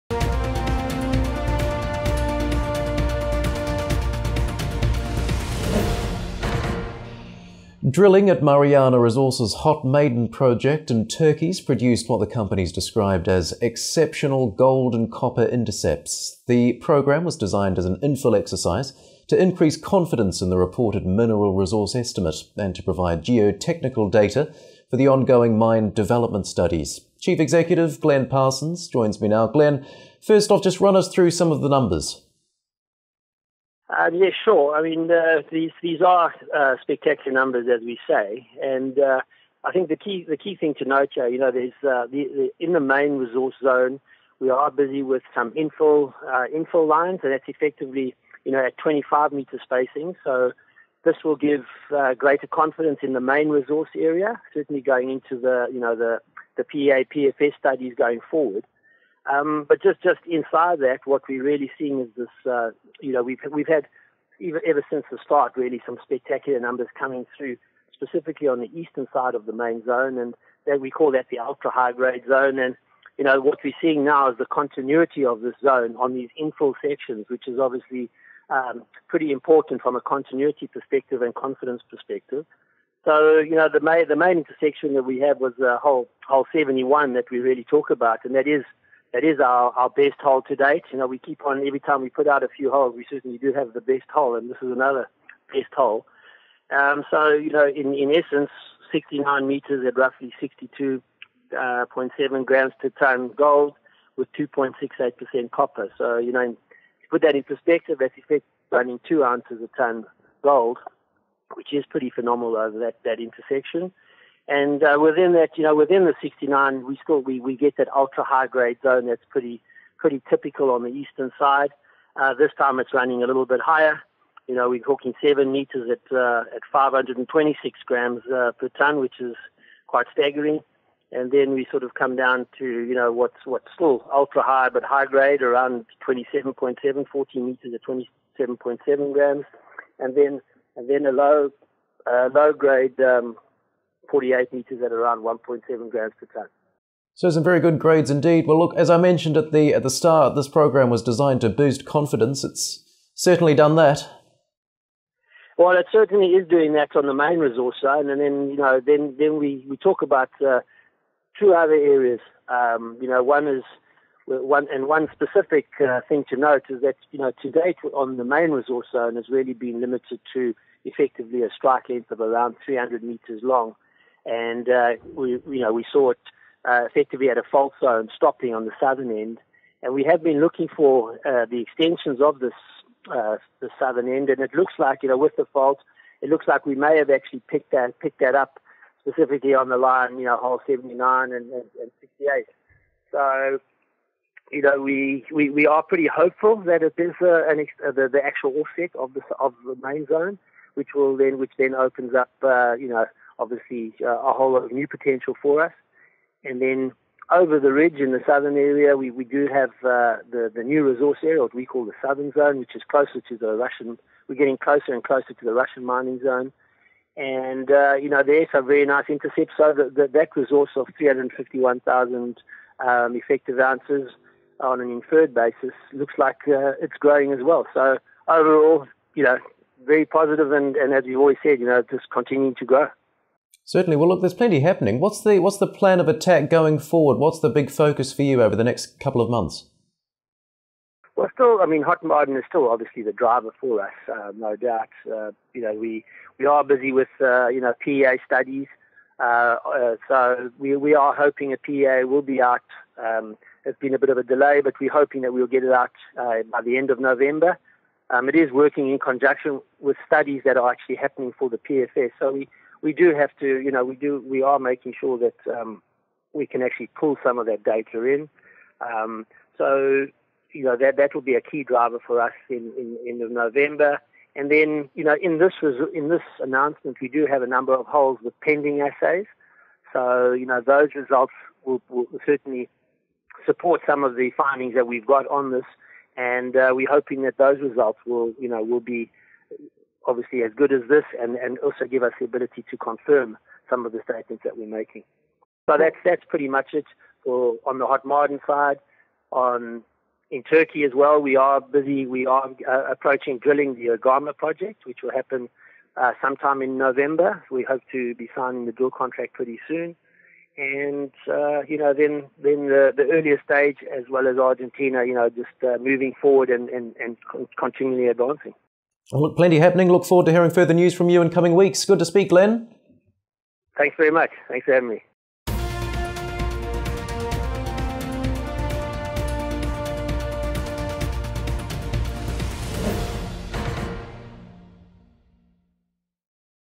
Mariana Resources does it again at Hot Maden
Glen Parsons, chief executive officer of Mariana Resources (LON:MARL, CVE:MARL, CVE:MRA), was understandably in ebullient mood after the gold explorer unveiled “exceptional” gold and copper intercepts at its Hot Maden project in Turkey.
Parson said the latest round of infill drilling has produced “spectacular results”, but he did not seem overly surprised, as “we’ve had them from the start”.
“What we are seeing now is the continuity of the zone,” he told Proactive Investors.
The latest drilling included the best drill hole to date, hole 72, and Parsons said “every time we put out an announcement, we have a new best hole to date”.
The hole contained a seven-metre section starting at 210 metres grading a bonanza 526 grams per tonne of gold and 3.28% of copper.
Mariana owns 30% of Hot Maden, with local partner Lidya holding the remainder.